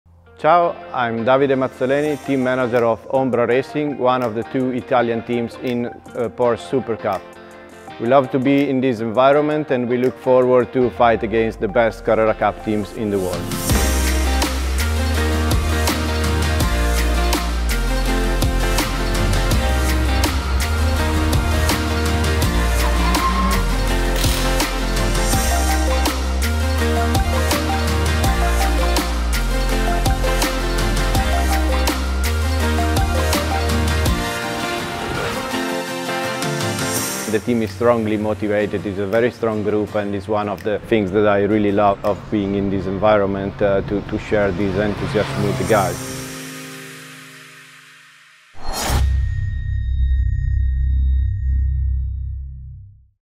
Get to know Team Ombra Racing
Our Team Up series concludes with Ombra Racing , an Italian racing team with a rich history! Under the leadership of Davide Mazzoleni, the team is fully prepared to take on the challenges at the top of the Porsche one-make series for the second year in a row. Want to know more about their plans for the season? Check out the link below for a full interview with their team manager and get ready to see them on track soon 👀